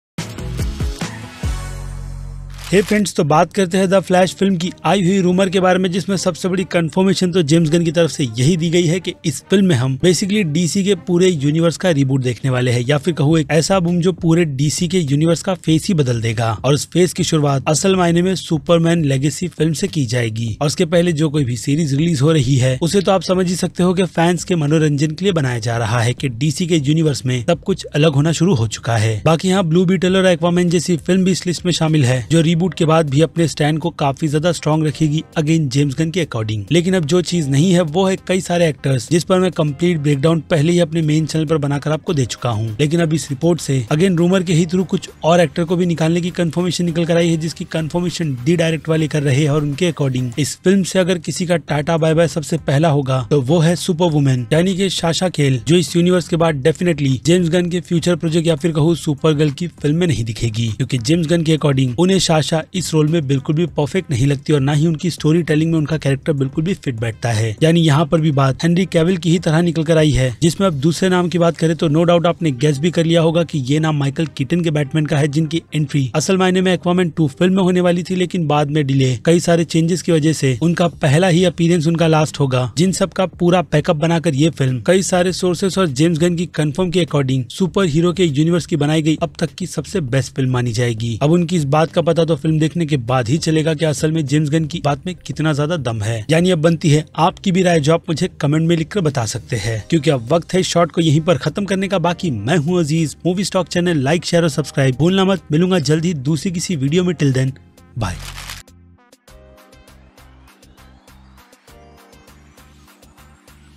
The Flash Movie Removes 2 More Superheroes from Ending?
Hello friend, today I am going to talk about the flash film updates in this video.